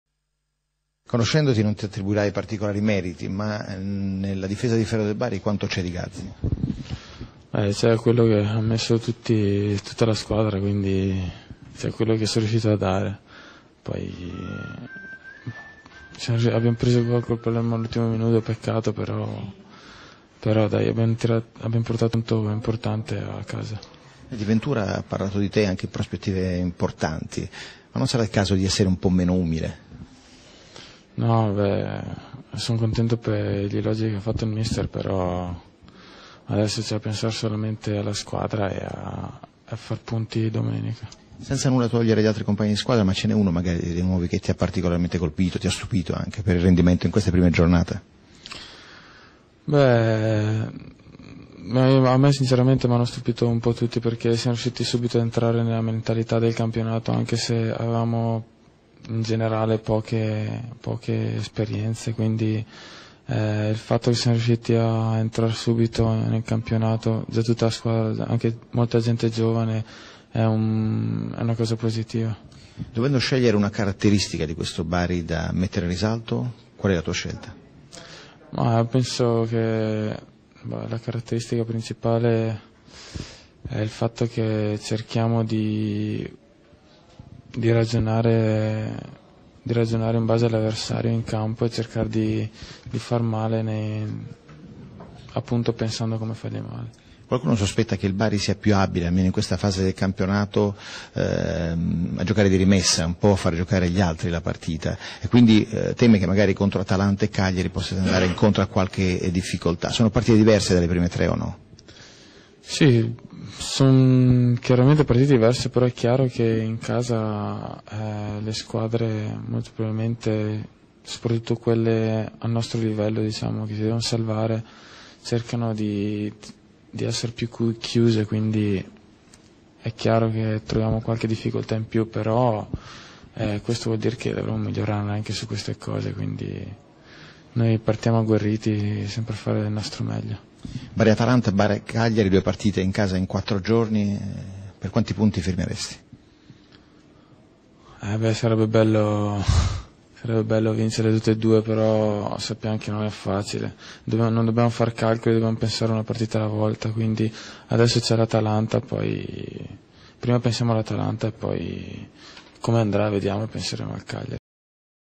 Intervista a Alessandro Gazzi
Intervista a Alessandro Gazzi(Centrocampista del Bari), intervisato in sala stampa alla fine dell' allenamento di stamane.